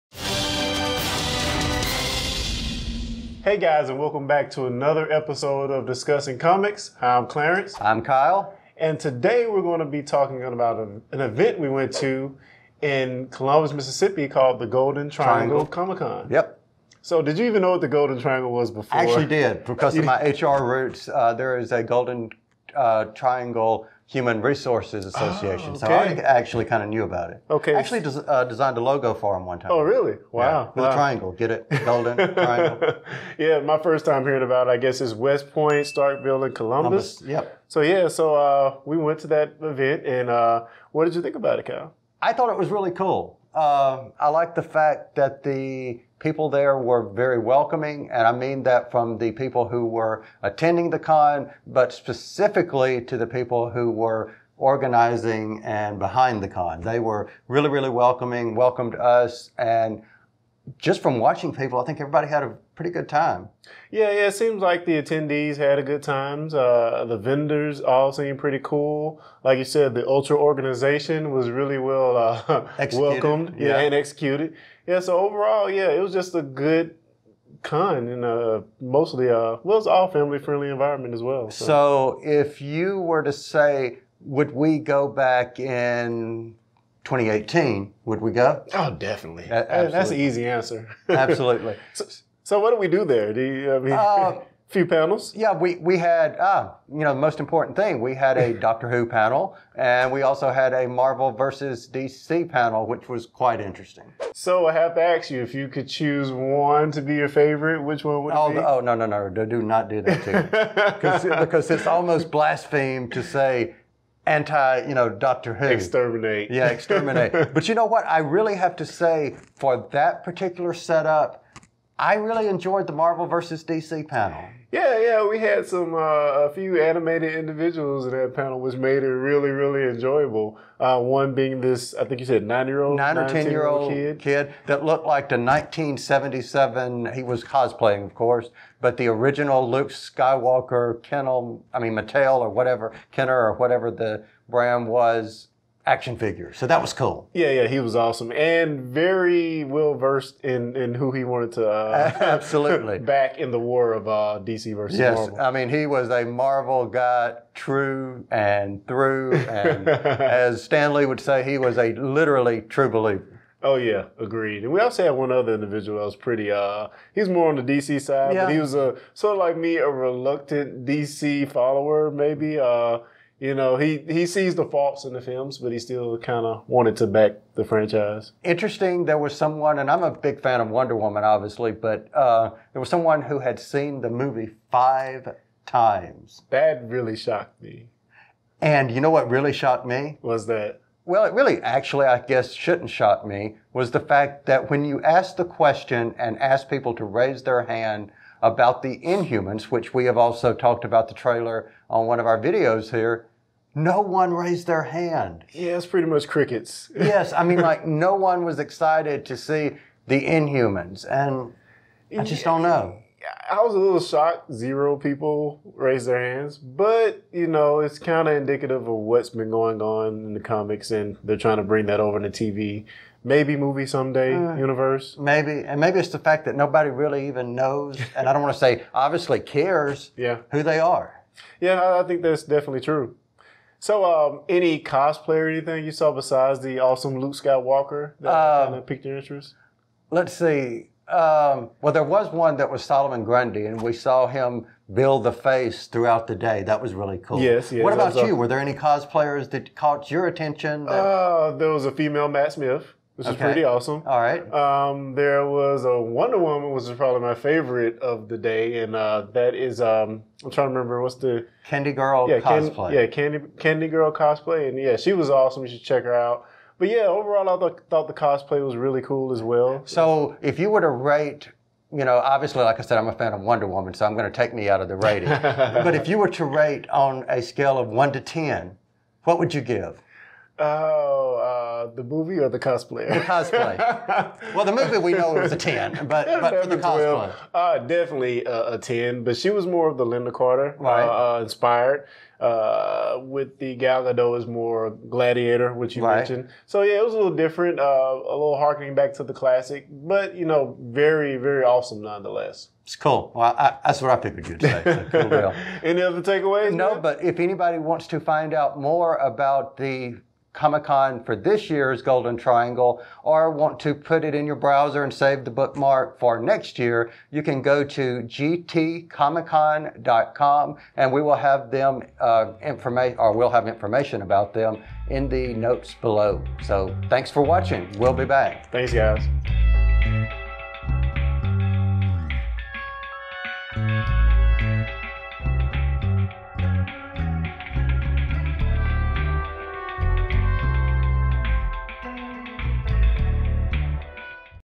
Golden Triangle Comic Con Recap | Discussing Comics
We recap our experience at the 2017 Golden Triangle Comic Con! Hosted by Kyle Jones and Clarence Brown.

About The Golden Triangle Comic Con

The Golden Triangle Comic Con (or GTCC afterwards) was held on Saturday, August 19, 2017 in Columbus, Mississippi on the second floor of the Trotter Center. For more information about the event and guest, go